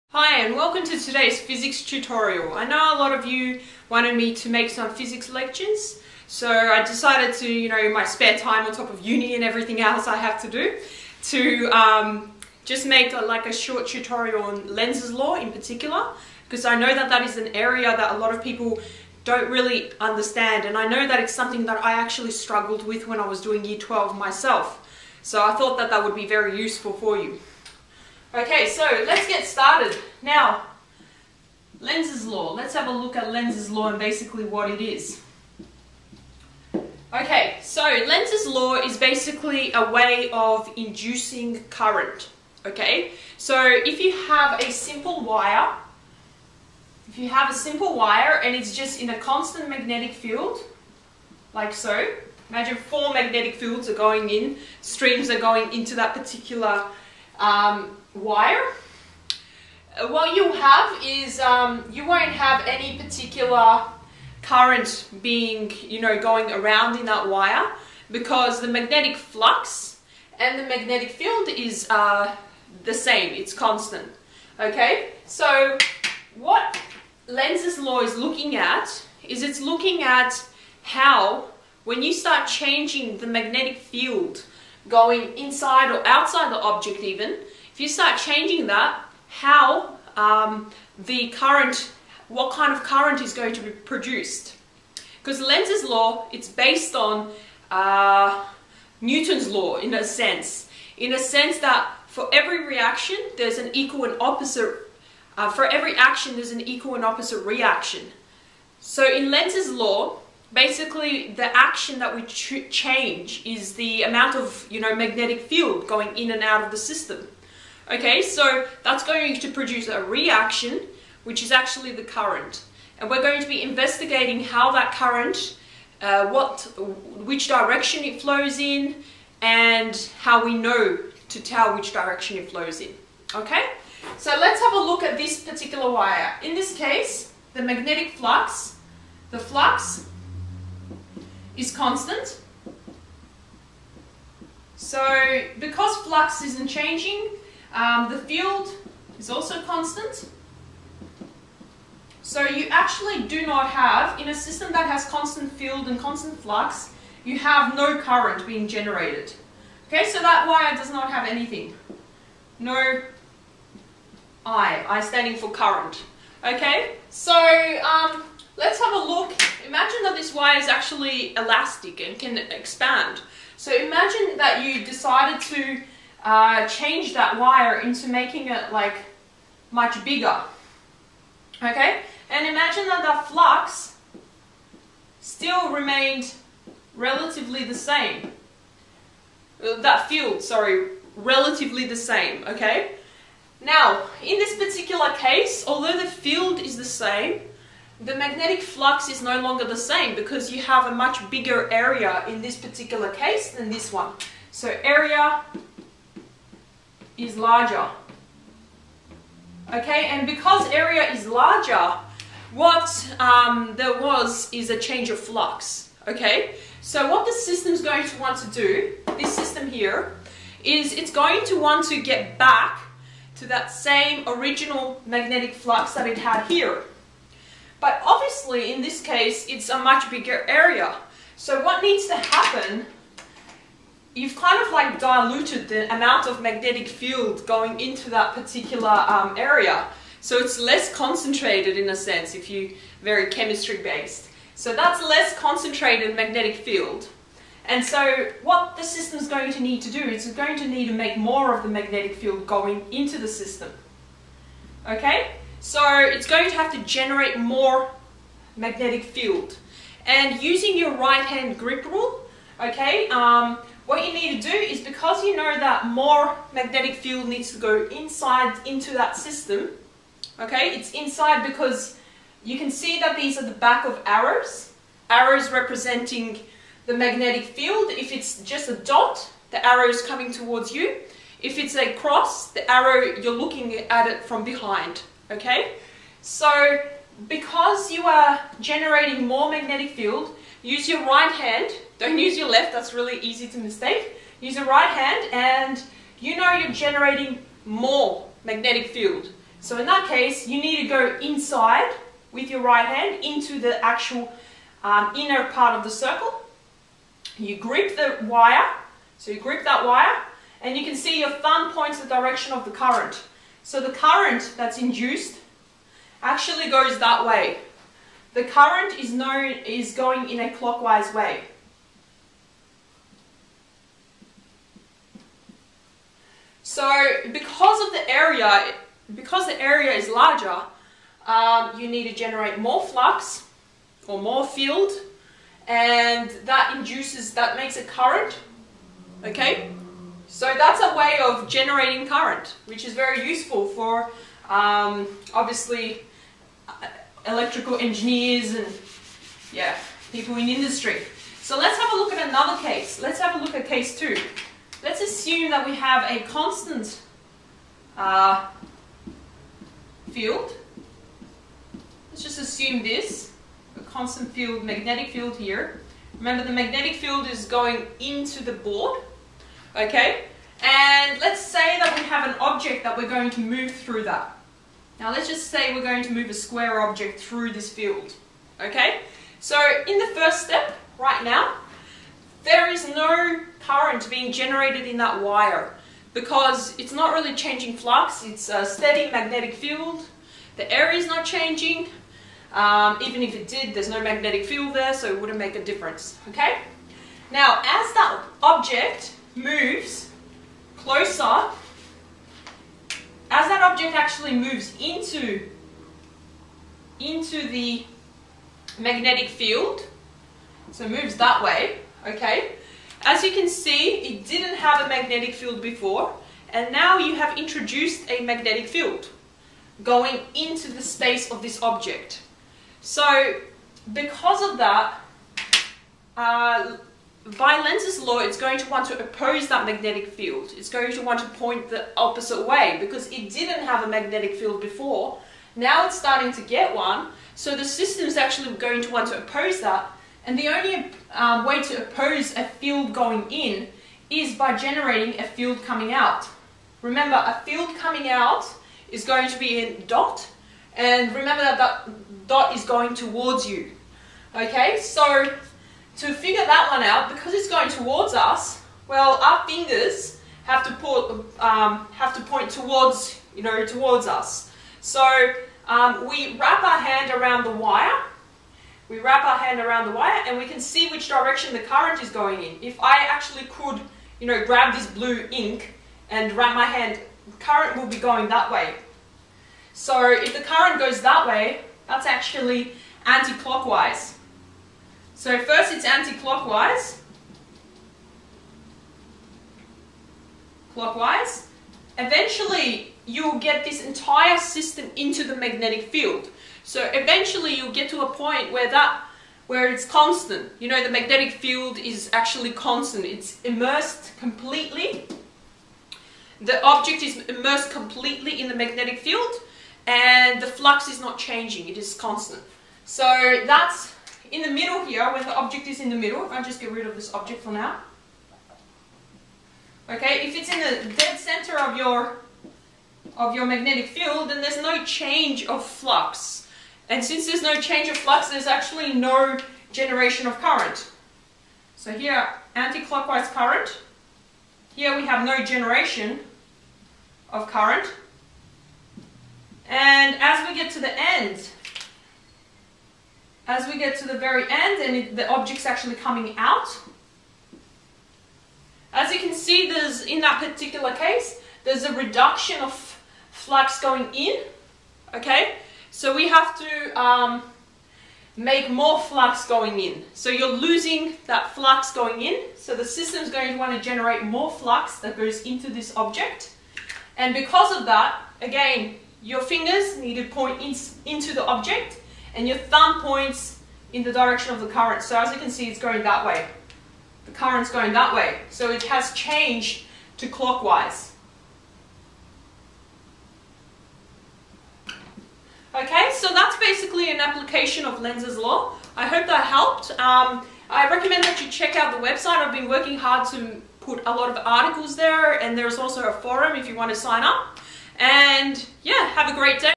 Academy Plus; VCE Physics - Lenz's Law
What is Lenz's Law and why do we used it? Also how do you work out the induced current? Enjoy